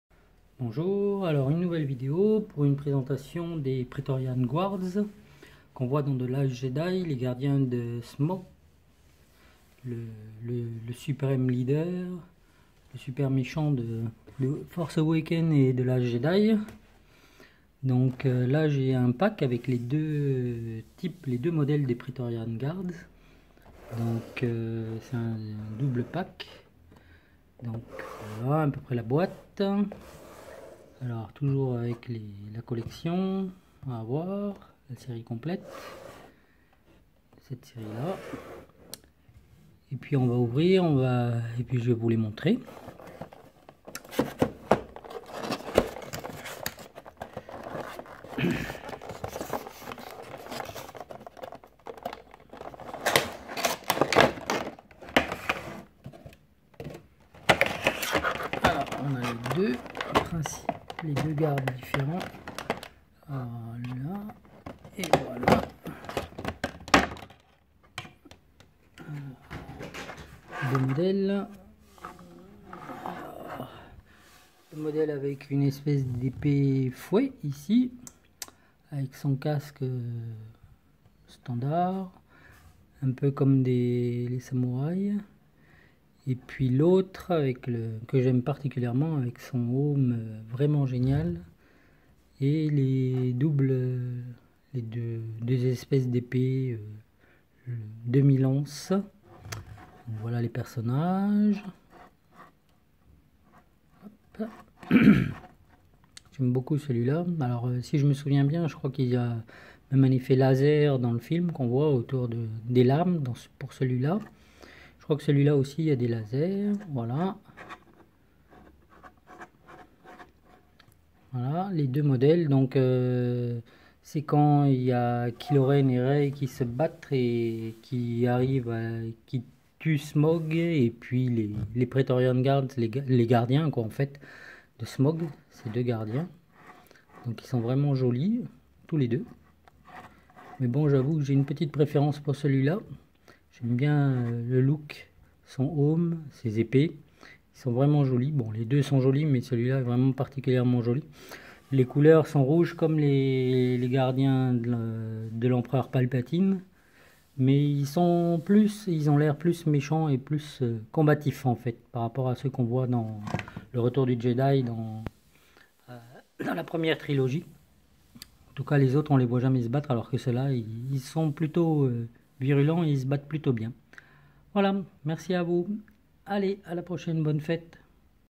Présentation Française Funko Pop Star Wars Praetorian Guards 2 Pack
Présentation en français
Funko pop movie
Star wars the last jedi episode 8
Praetorian guards
Very nice very impressed by them look like old samurai